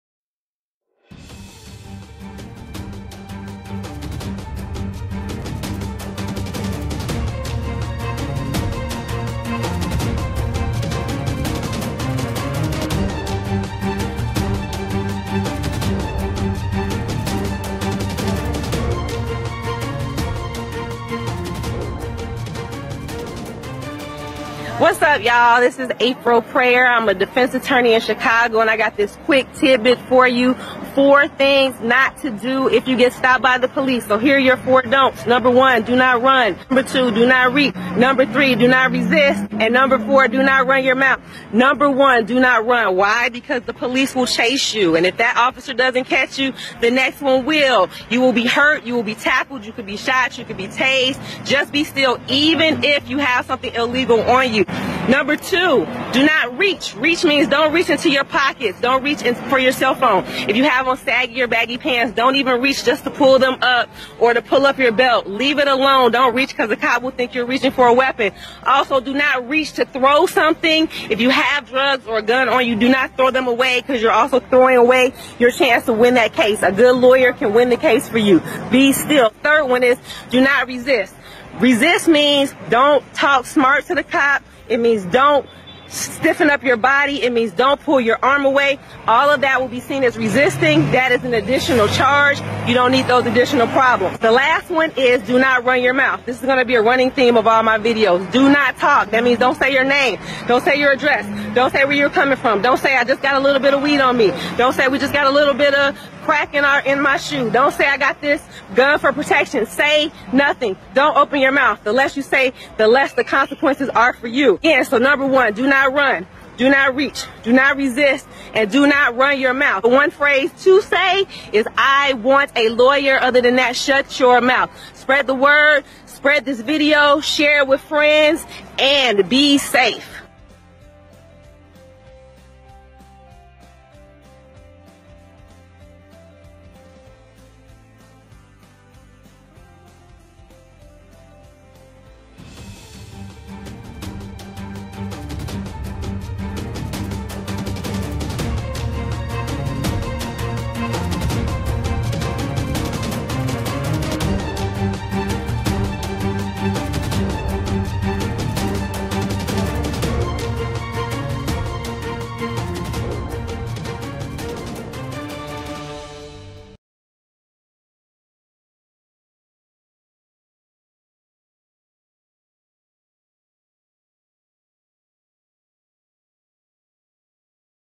4 Things Not To Do When Stopped By The Police
Defense Attorney April Preyar wants you to stop doing these 4 things when stopped by the police while walking!

*April is licensed to practice law in Illinois. If you live elsewhere, consult a licensed attorney in your state.

Apparel and Merchandise inspired by BLACK ANGEL MEDIA.